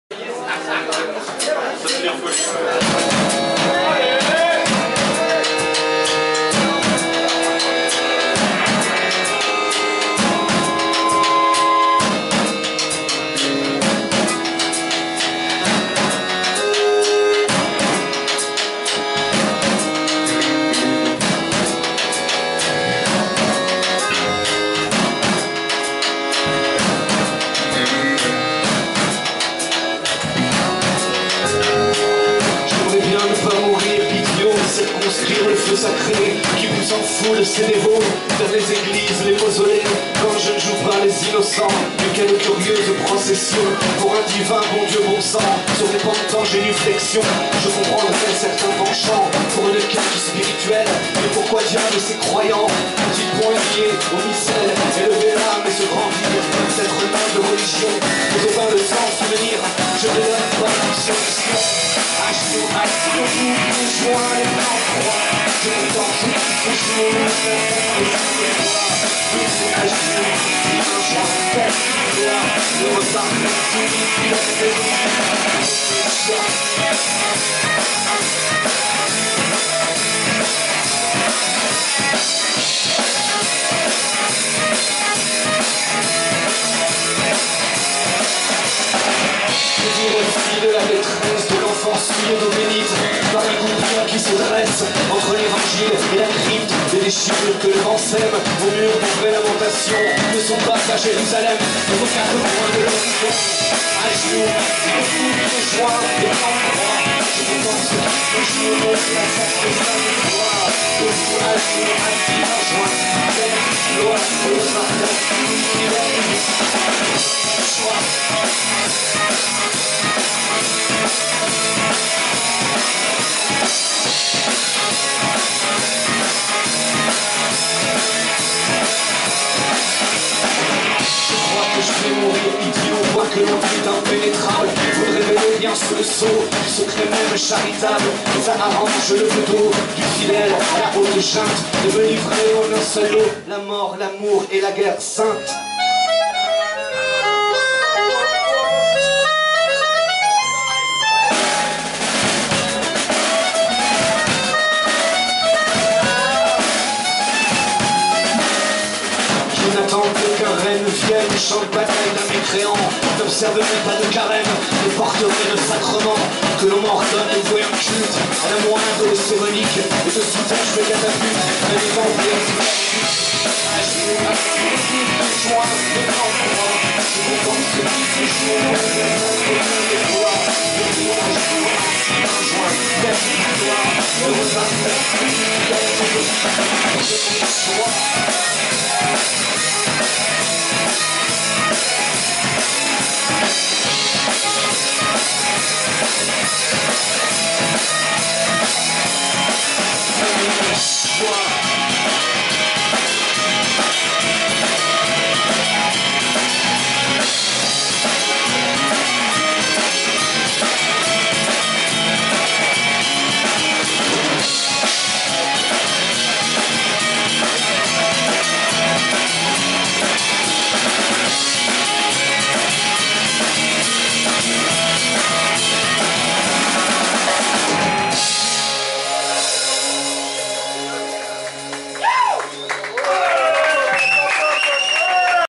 Lamilca
En concert au Perceval Geoffrey Bar à Montluçon